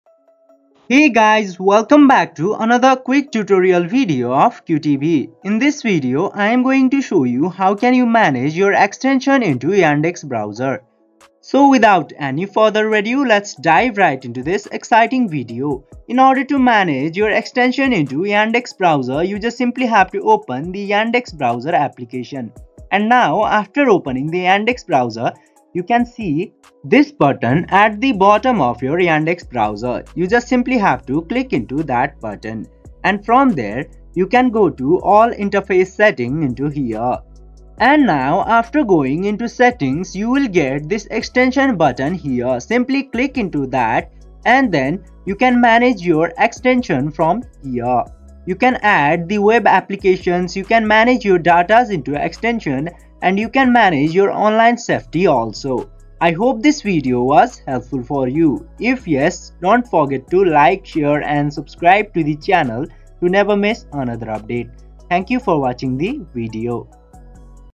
How to Manage Extensions in Yandex Browser 2024?
Whether you're looking to boost productivity, enhance security, or customize your browsing experience, extensions play a crucial role in shaping how you interact with the web. In this video, we'll walk you through everything you need to know about managing extensions in Yandex Browser, from installing and removing them to customizing their settings to suit your needs.